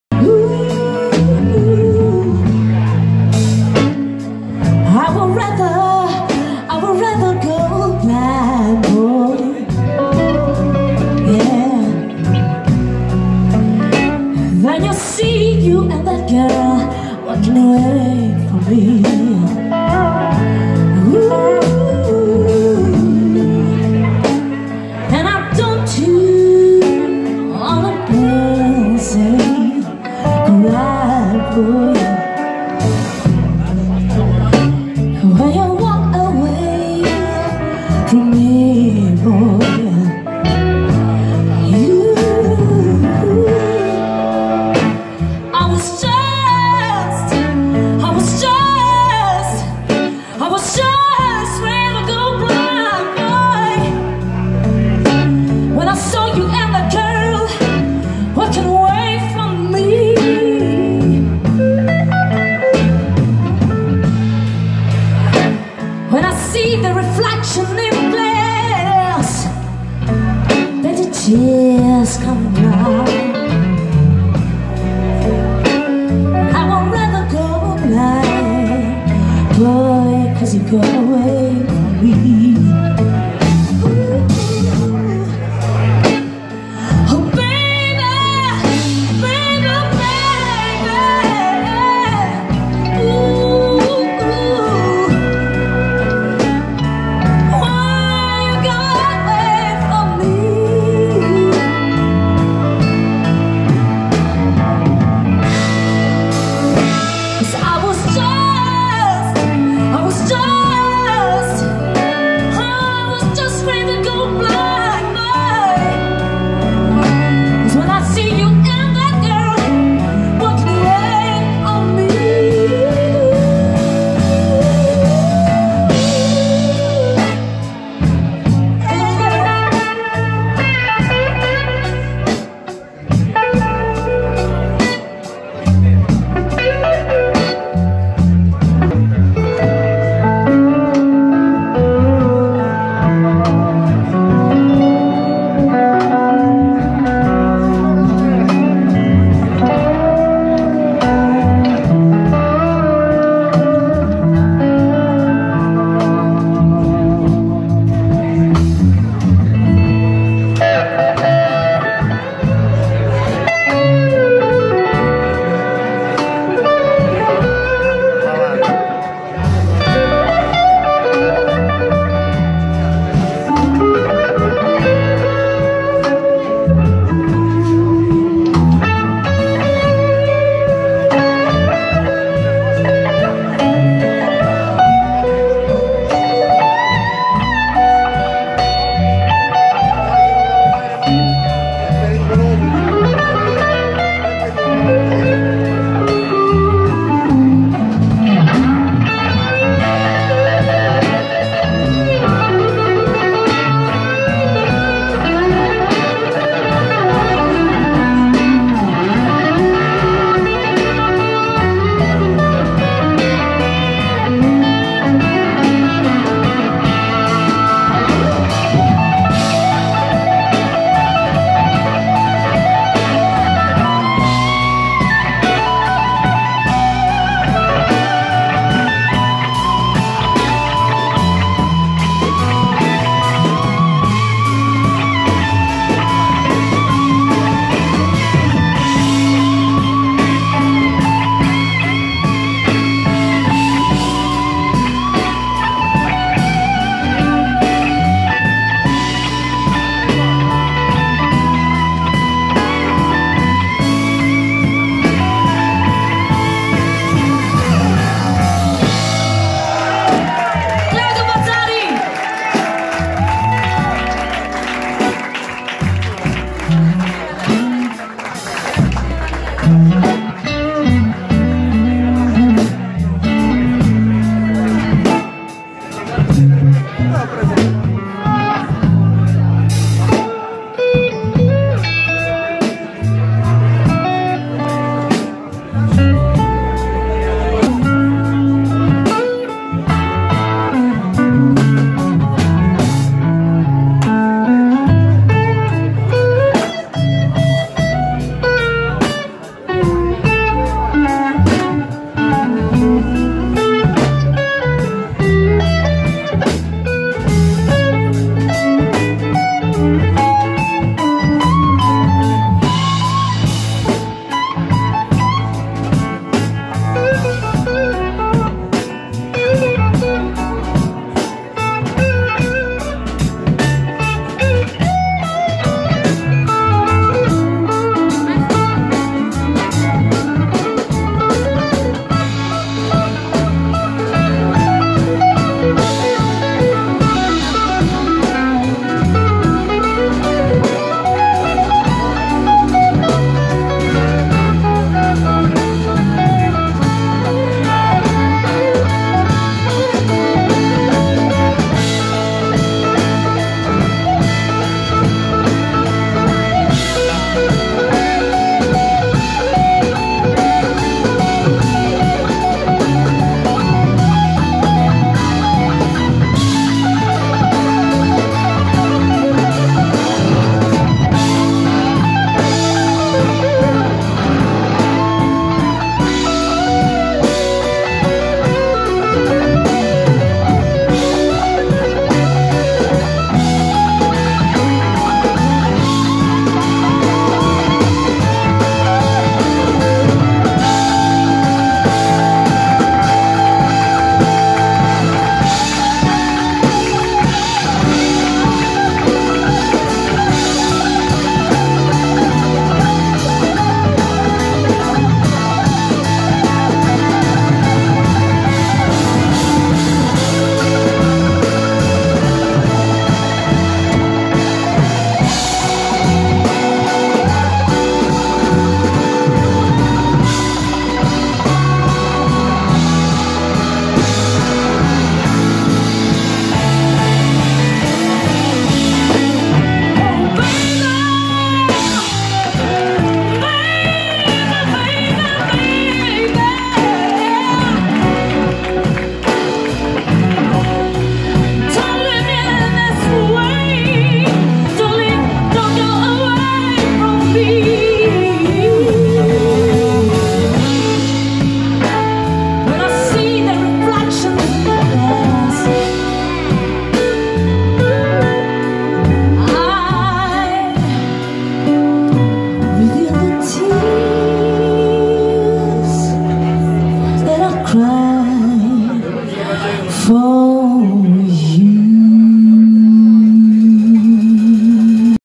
BLUES GUITAR HEROES ft. ISABELLA CASUCCI - OSVALDO DiDIO
Stupendo concerto  dei BLUES GUITAR HEROES con due ospiti d'eccezione : Osvaldo DiDIO  e Isabella Casucci